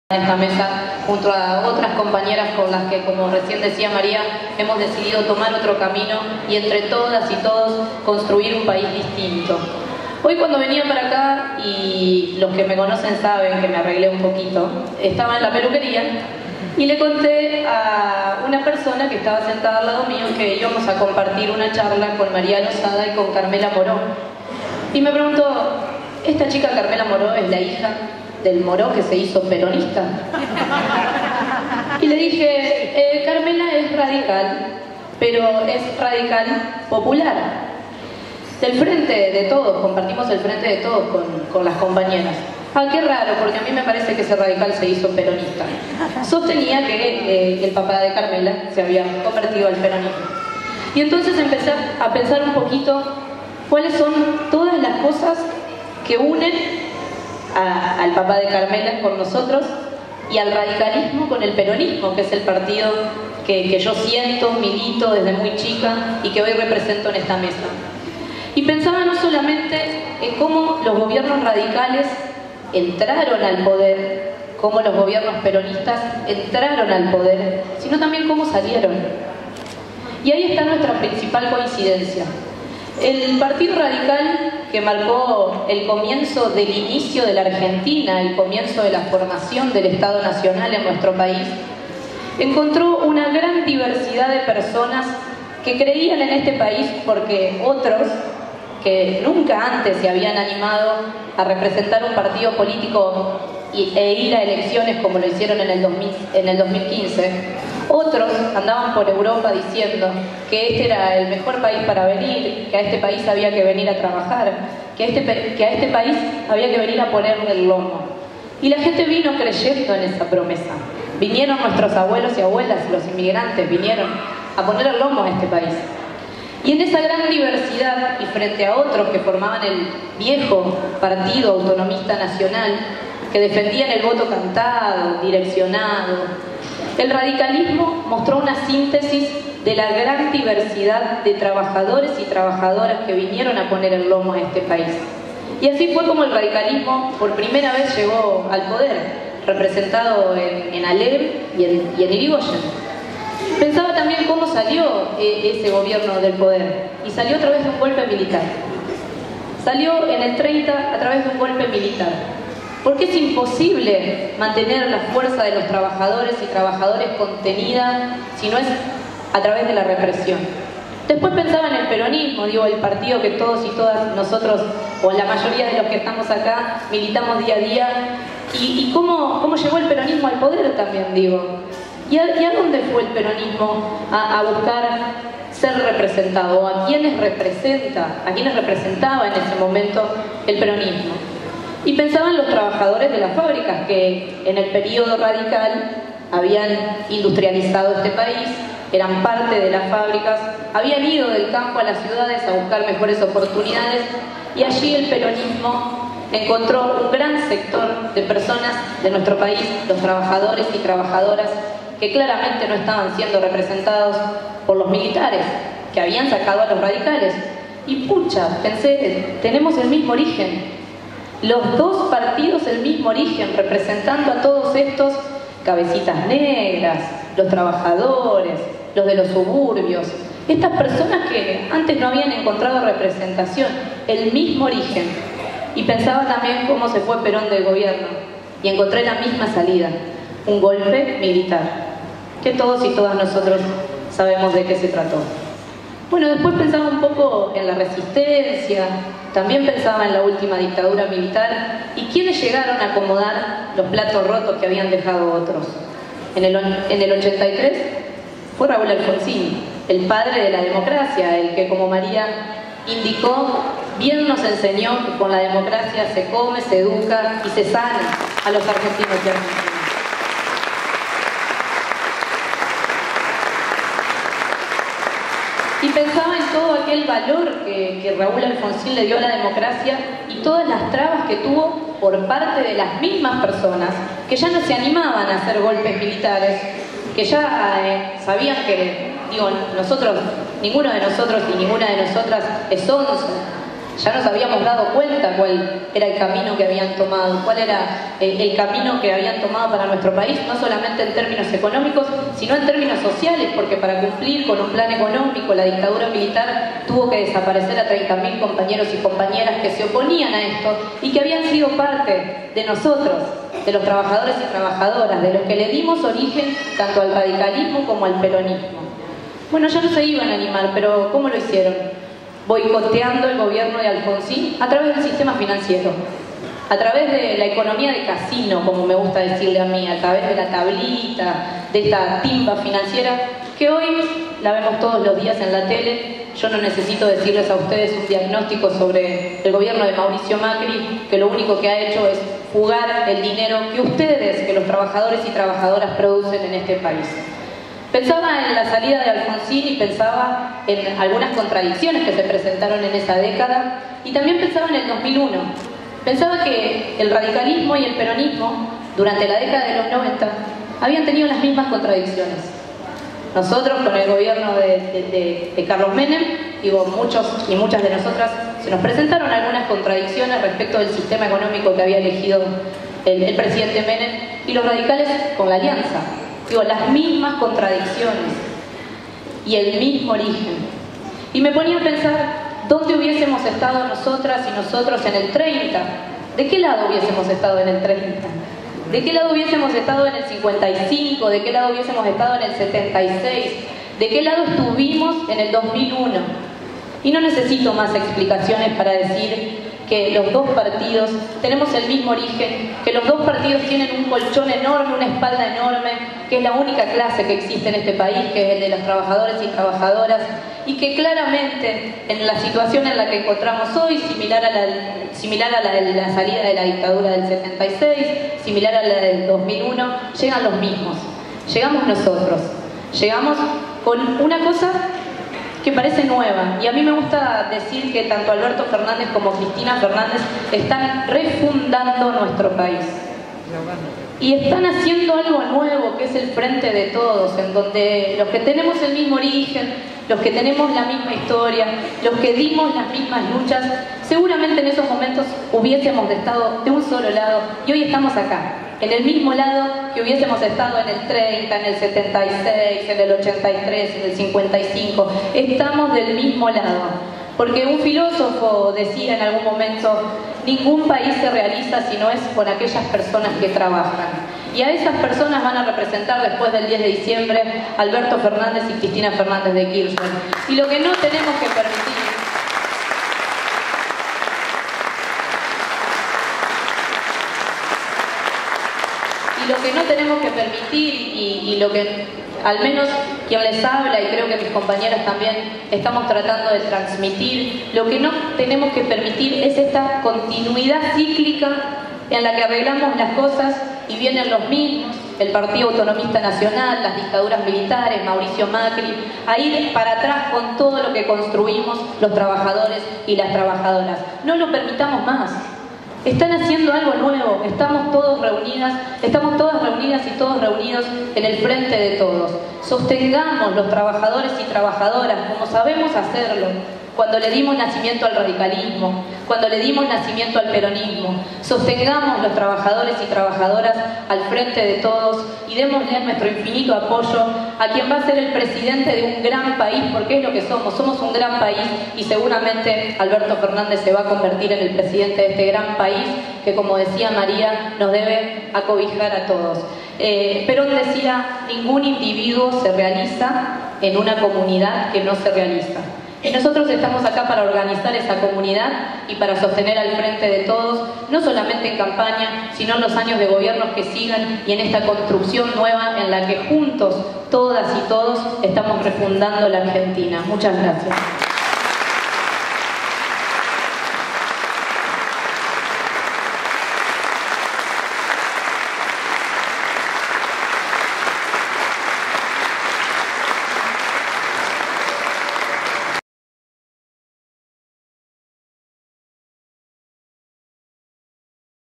“El rol de todos en la Argentina que se viene”
La jornada contó con la disertación de la Responsable técnica del Frente de Todos en Santa Fe y dirigente justicialista Natalí Bedini y las dirigentes radicales Carmela Moreau y María Losada.